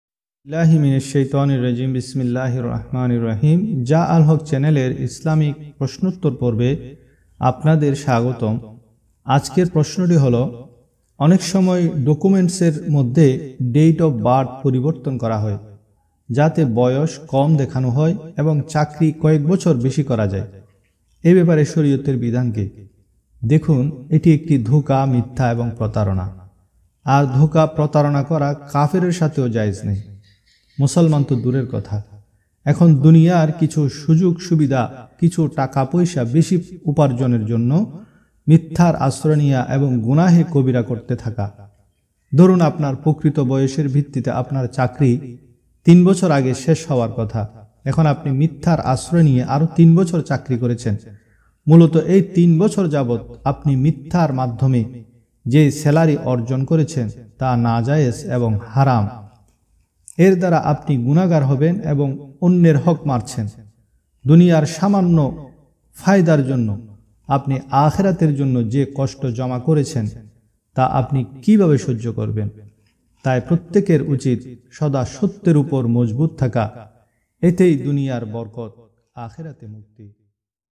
ডেইট অব বার্থ পরিবর্তন করা কি জায়েজ চাকরীর মেয়াদ বাড়ানোর জন্য? প্রশ্নোত্তর Sawal jawab jaalhaq
চাকরী বেশী করার জন্য ডেইট অব বার্থ পরিবর্তন করা কি জায়েজ
জা আল হক চ্যানেলের ইসলামী প্রশ্নোত্তর পর্বে আপনাদের স্বাগতম আজকের প্রথম প্রশ্ন হল
-অনেক সময় ডকুমেন্টস এর মধ্যে ডেইট অব বার্থ পরিবর্তন করা হয়, যাতে বয়স কম দেখানো হয় এবং চাকরী কয়েক বছর বেশী করা যায়। এ ব্যপারে শরীয়তের বিধান কি?
-দেখুন এটি একটি ধোকা মিথ্যা ও প্রতারনা, আর ধোকা প্রতারনা করা কাফেরের সাথেও জায়েজ নাই। এখন দুনিয়ার কিছু সুযোগ সুবিধা কিছু টাকা পয়সা অর্জনের জন্য মিথ্যার আশ্রয় নেয়া এবং গুনাহে কবিরা করতে থাকা। ধরুন আপনার প্রকৃত বয়সের ভিত্তিতে আপনার চাকরী ৩ বছর আগে শেষ হওযার কথা এখন আপনি মিথ্যার আশ্রয় নিয়ে আরো ৩ বছর চাকরী করেছেন, মুলত এই ৩ বছর যাবত আপনি মিথ্যার মাধ্যমে যে সেলারি হাসিল করেছেন তা নাজায়েজ সেলারি গ্রহণ করেছেন। এর দ্বারা আপনি গুনাহগারও হবেন এবং অন্যের হক মারছেন। দুনিয়ার সামান্য ফায়দার জন্য আপনি আখেরাতের জন্য যে কষ্ট জমা করেছেন তা আপনি কিভাবে সহ্য করবেন? তাই প্রত্যেকের উচিত সদা সত্যের উপর মজবুত থাকা এতেই দুনিয়ার বরকত আখেরাতে মুক্তি।